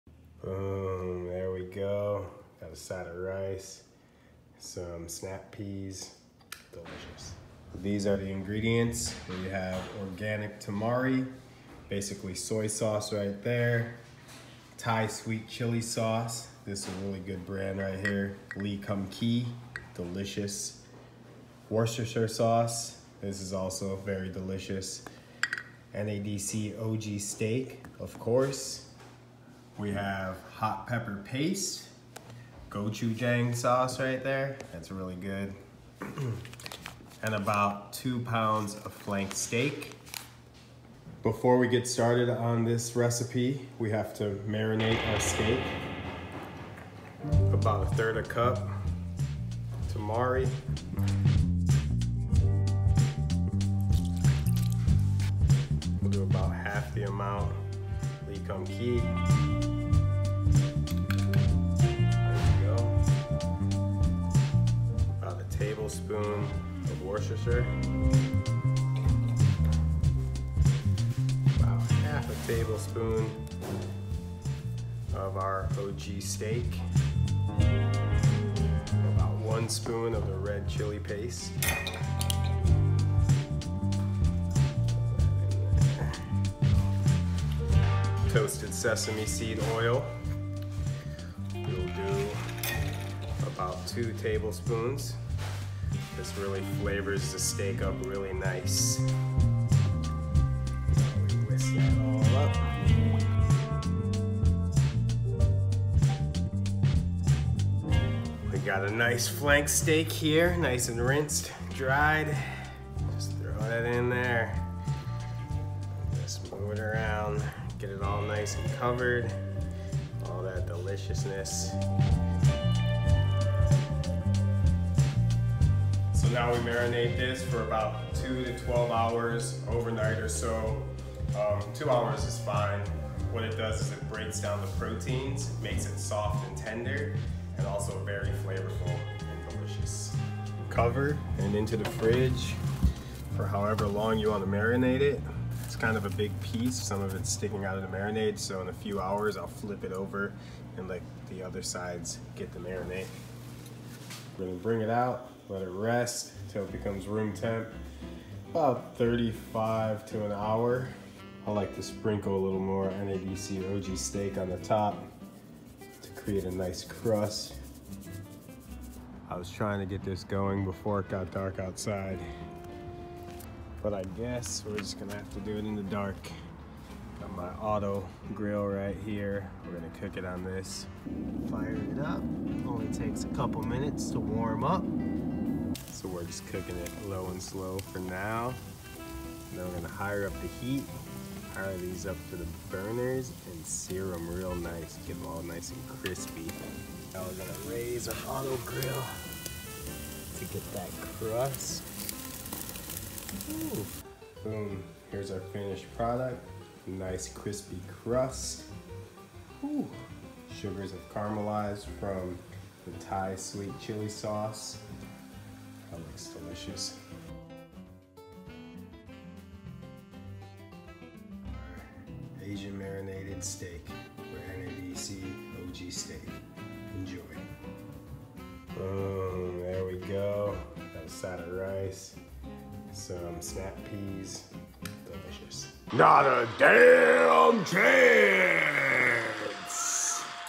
Daves NADC Asian Marinated steak
for NADC spice rubs and gear!

Ingredients:

2 Lb Flank steak or Bohemian steak
Tamari or soy sauce, 1/3 cup
Lee Kum Kee Thai chili sauce, 1/4 cup or so
Worcester sauce, 1 Tablespoon
Gochujang sauce (hot pepper paste) 1 Tablespoon
NADC OG Steak, 1/2 tablespoon
toasted sesame seed oil, 2 Tablespoon

The Night before
1. Make your marinade.
1/3 cup soy sauce, 1/4 cup Thai chili sauce, 1 Tbsp Worcester sauce, 1/2 Tbsp NADC OG Steak, 1 Tbsp Gochujang, 2 Tbsp toasted sesame oil, Whisk well.
2. Rinse and dry the steak, submerge the steak in the marinade and cover. Place in your fridge and let rest for 2-12 hours. The longer the better.
3.Once marinated, take the steak out of the fridge, take the steak out of the marinade and let rest until the steak is room temp. This takes about 30-45 minutes. This also insures an even cook.
4. Time to cook your steak. Heat up your grill or in this case I used my Otto Grill. The steak can take anywhere from 3-7 minutes a side depending on the size of your steak and how you like it cooked. I like to cook my flank steak to about and internal temp of 130F Which is medium. I always suggest using a digital thermometer, you can get these anywhere for cheap.
5. Once the steak is cooked to your liking take it out and let it rest for about 5 minutes, slice and enjoy!

Follow on Instagram: @neenwilliams and @nadcofficial for daily updates.